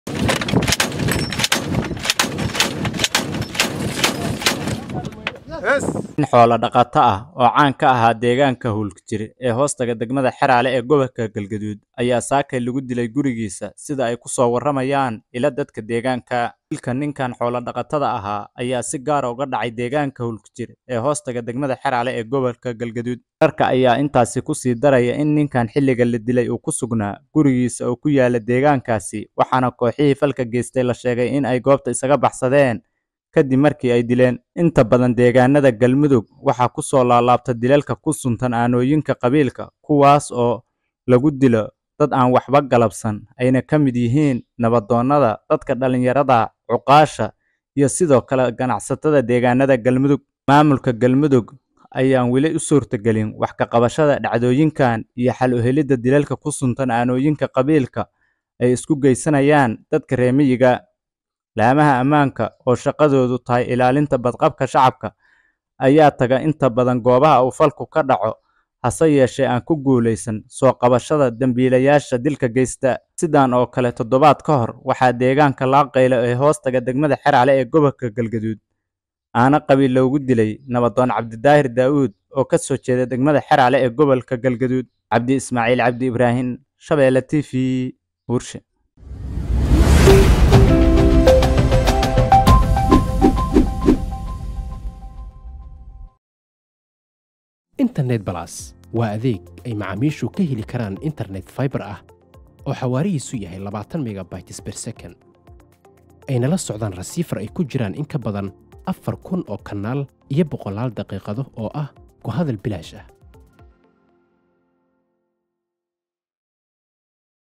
Nin Xoolo dhaqato ahaa oo aano qabiil loogu dilay Deegaanka Huul-kujir ee hoos taga Degmada.........
Nin Xoolo dhaqato ahaa oo aano qabiil loogu dilay Deegaanka Huul-kujir ee hoos taga Degmada Xeraale ee gobolka galgaduud.